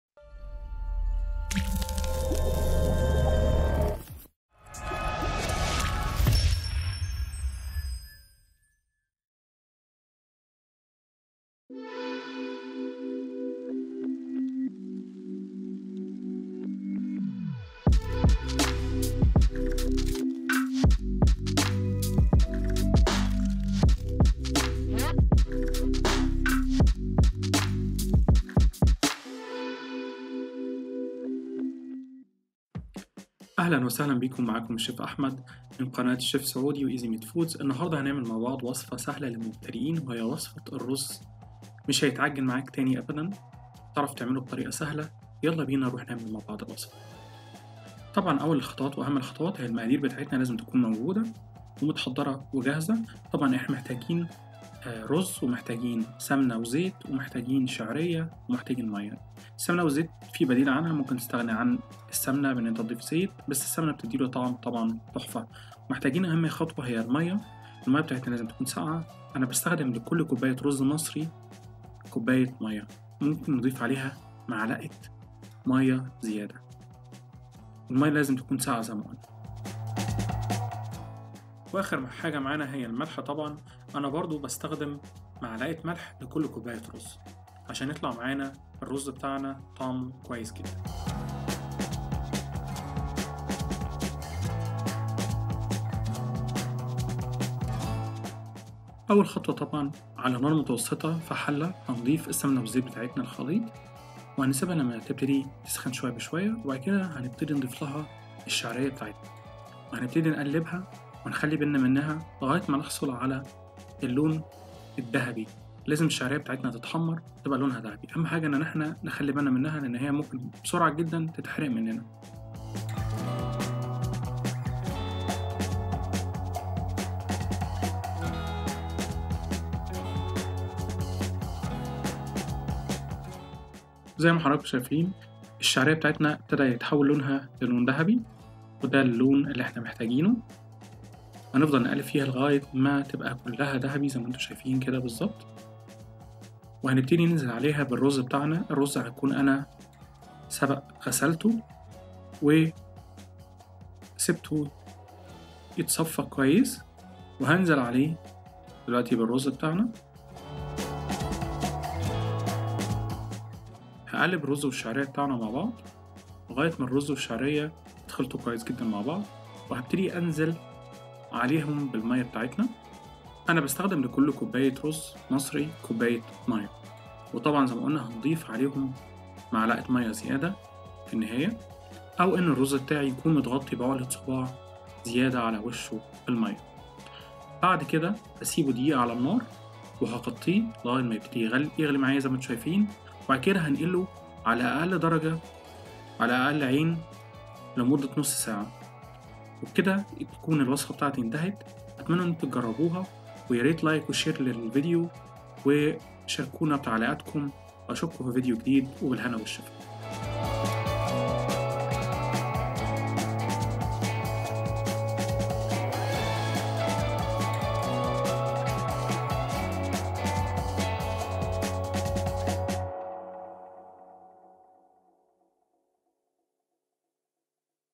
Egyptian Rice / Rice for beginners ♥♥♥ رز مصري بالشعريه للمبتدين /رز مفلفل ♥♥♥♥
رز مصري بالشعريه / رز بالشعريه للمبتدين /رز مفلفل ومش معجن

هنتعلم طريقه عمل الرز المصري المفلفل علي اصوله للمبتديئن طريقه سهله وبسيطه مش هتخلي الرز يعجن معاكي او معاك
ده لينك صفحتنا علي الفيس بوك

المكونات
عدد 2 كوب رز مصري مغسول مسبقا
عدد ملعقه ونصف سمن
عدد نصف ملعقه زيت نباتي
عدد نصف كوب شعريه
عدد كوب ماء لكل كوب رز يعني 2 كوب ماء
عدد 2 ملعقه ملح لكل كوب رز ملعقه ملح
التسويه لمده نصف ساعه علي اقل اصغر عين بوتاجاز علي اقل درجه
Egyptian Rice / Rice  for beginners

We will learn how to make Egyptian rice for the beginners in an easy and simple way that will not make the rice Sticky with you
Components
2 cups pre-washed Egyptian rice
1 1/2 tablespoons of ghee
Half a tablespoons of vegetable oil
half a cup of small Noodles
1 cup water per cup rice means 2 cups of water
2 tablespoons salt, per cup of rice 1 tablespoon salt
cooking for half an hour at the very lowest the smallest Stove eye on the lowest Temperature